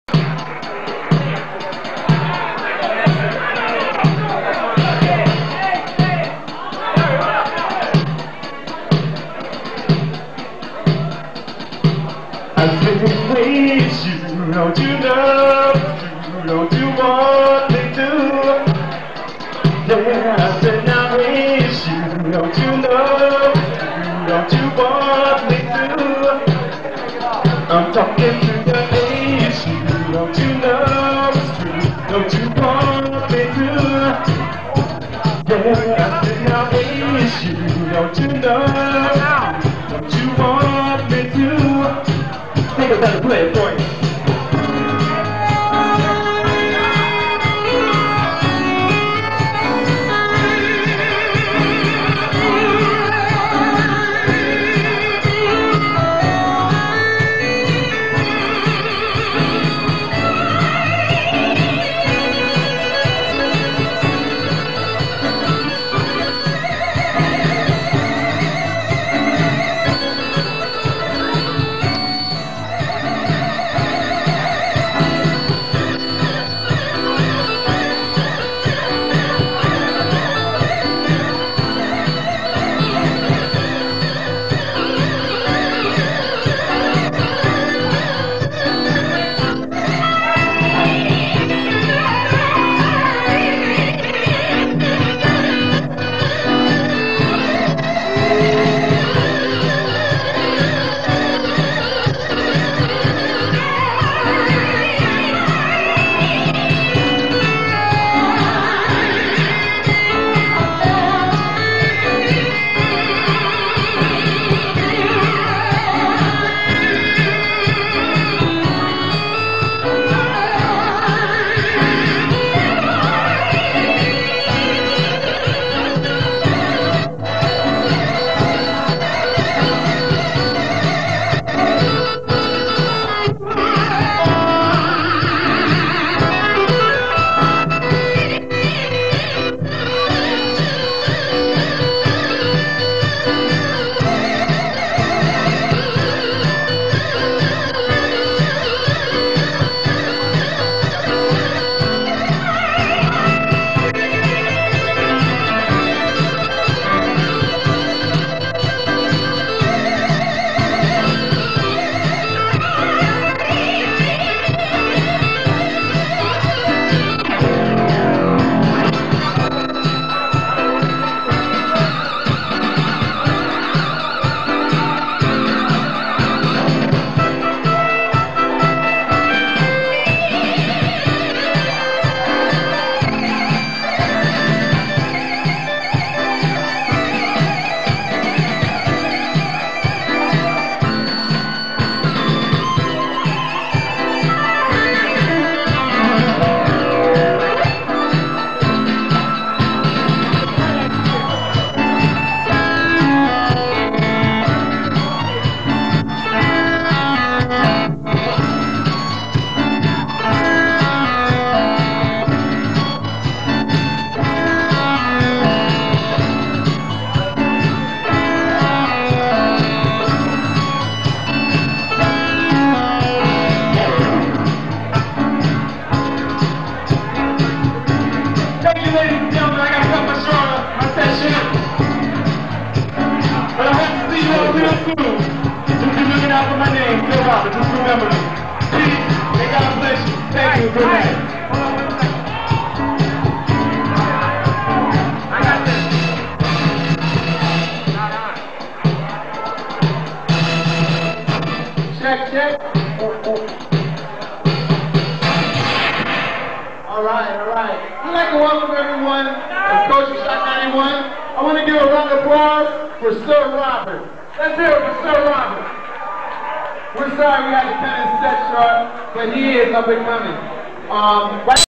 SIR ROBERT @ FROZEN PARADISE 1992 CLIP4.MP4
Live @ Frozen Paradise 1992
Lead synth solo
Korg RK100 midi into a Yamaha DX100.Backing track ESQ1